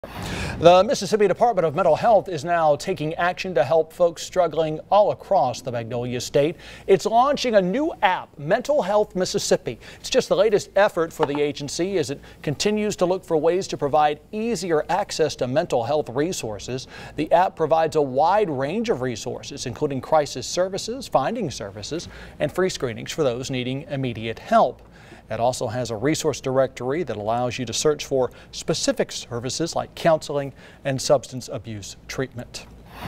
Mississippi Department of Mental Health announces mobile app
The Mississippi Department of Mental Health announced a new mobile app.